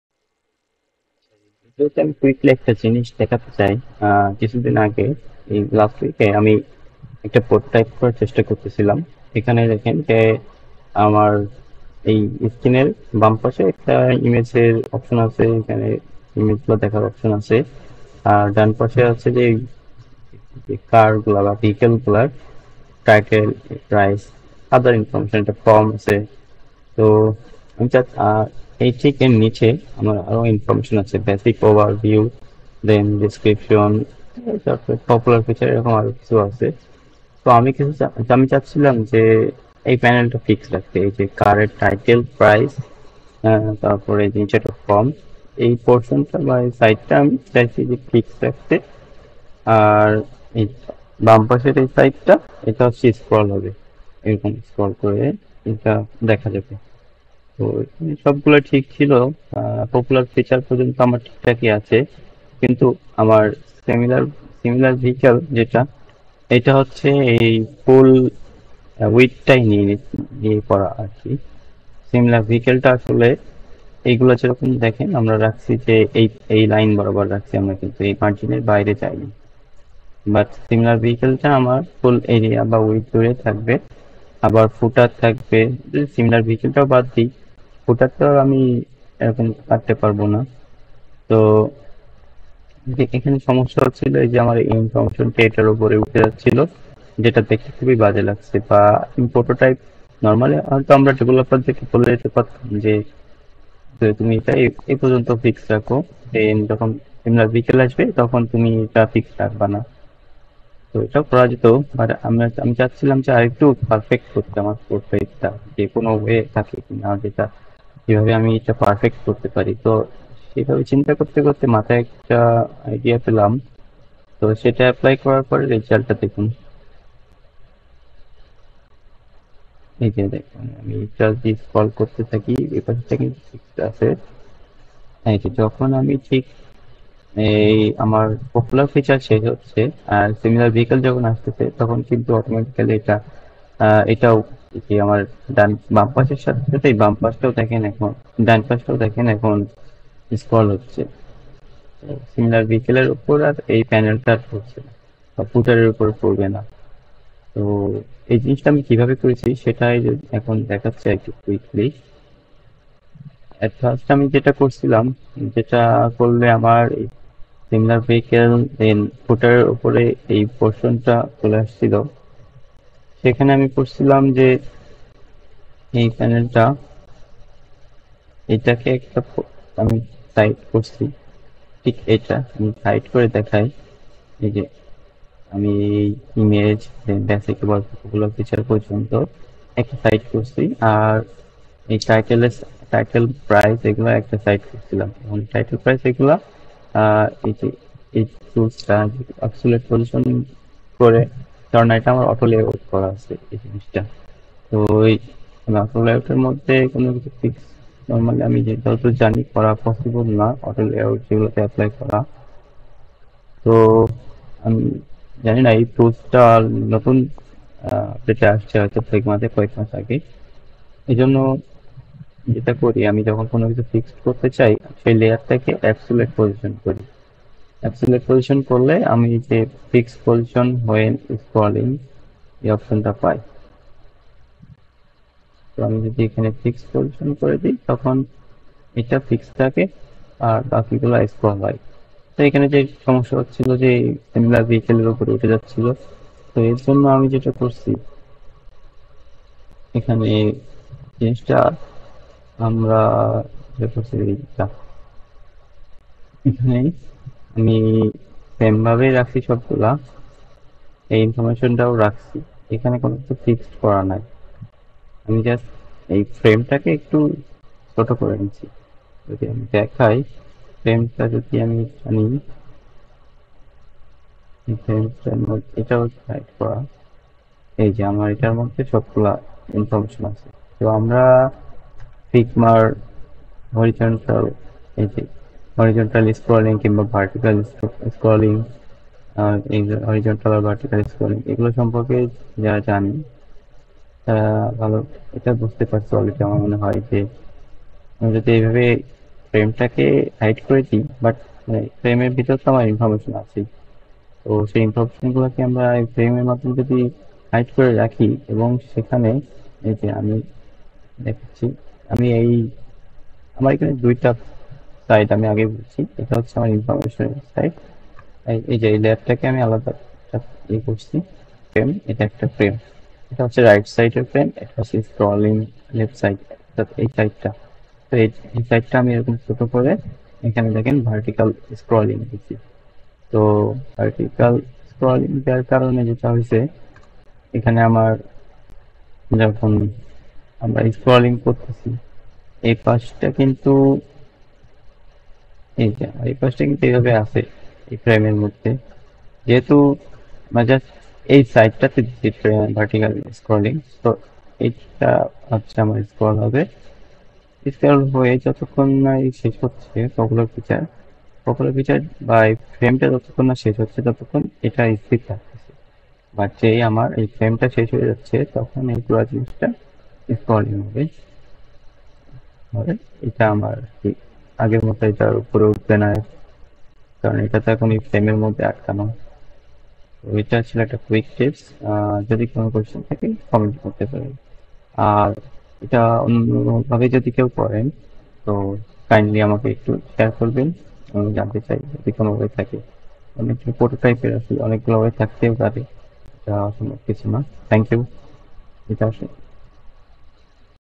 Scrolling keeping one side fixed | Figma Prototype
As-salamu Alaykum,
In this video I have tried to show a prototype or animation using figma. I think this trick can be used in Adobe Xd too. Enjoy!